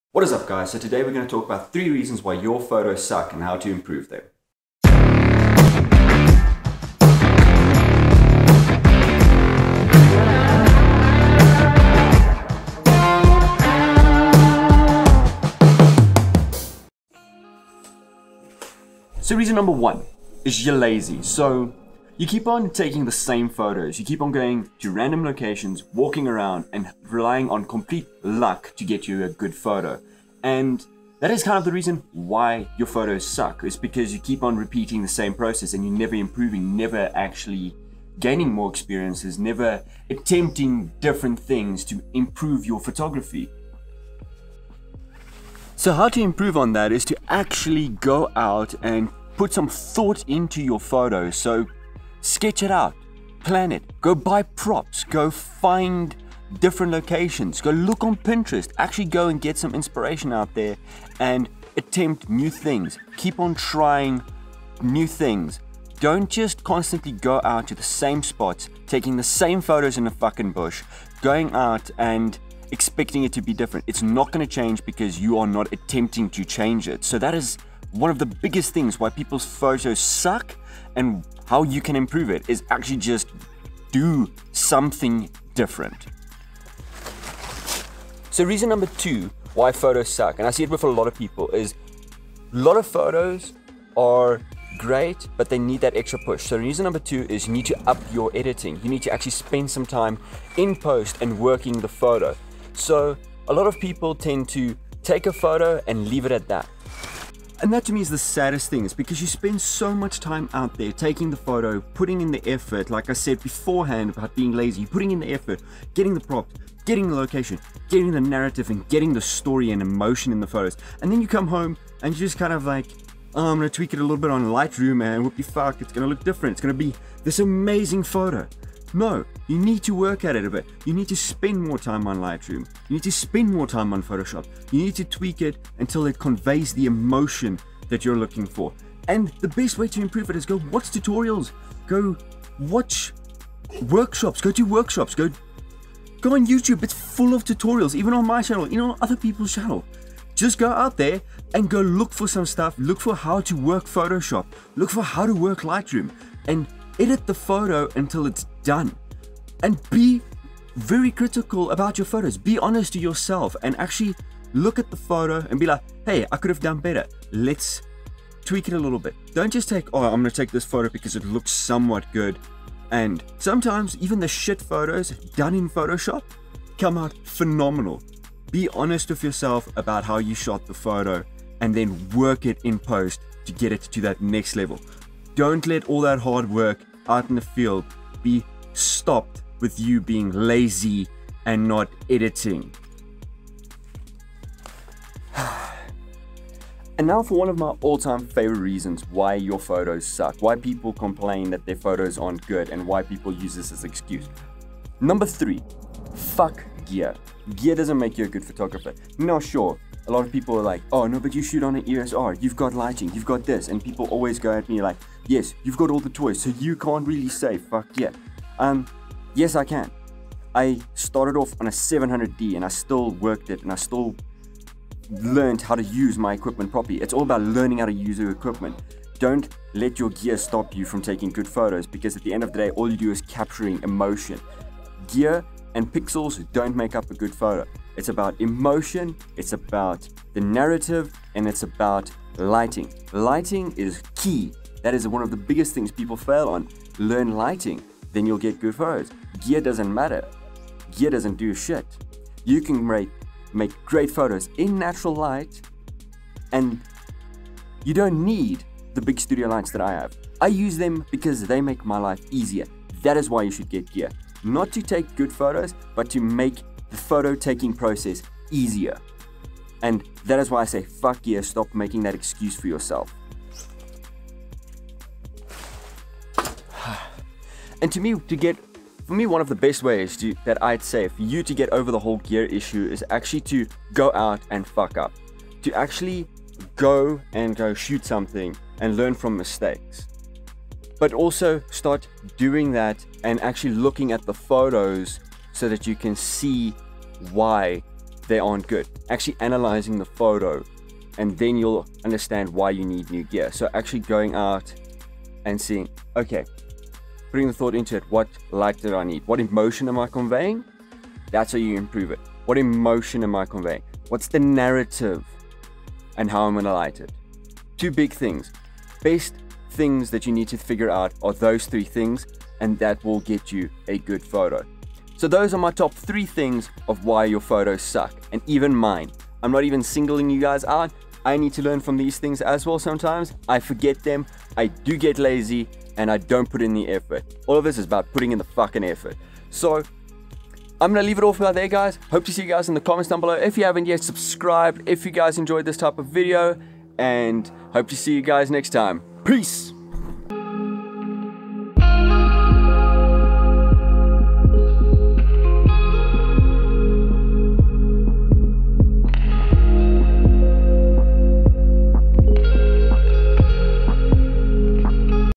Tips to improve your photography
This is gonna be hard to hear but these are the top reasons photos suck.

Don't fall in these traps during your photography career and you'll do great. Never compare and compete. Be creative and allow yourself to suck. GOD KNOW I HAVE.

My channel

TubeBuddy for all your Youtube Help get ranked quiker.